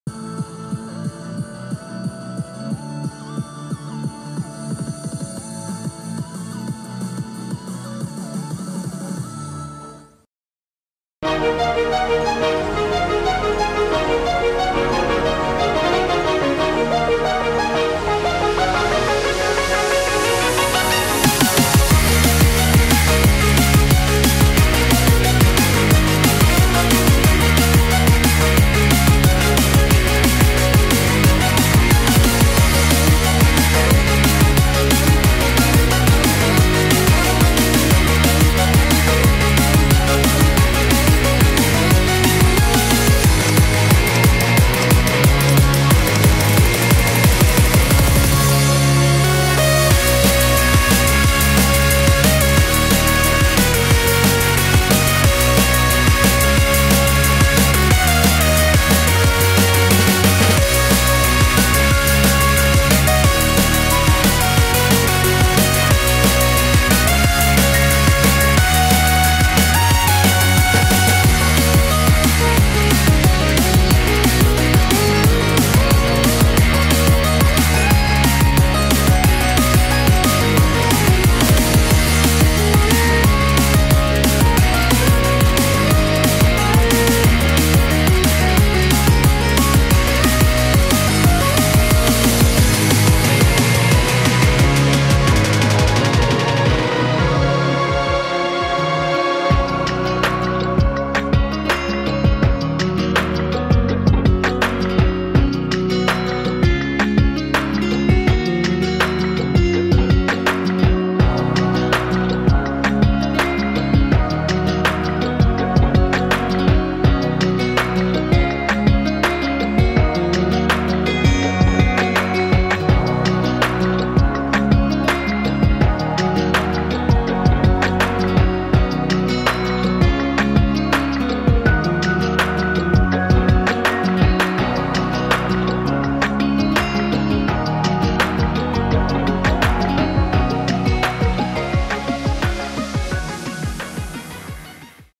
Drawing Yuji Uekawa Style Sonic!
This was so much fun to make, I always loved this style.

Remix by Silent Dreams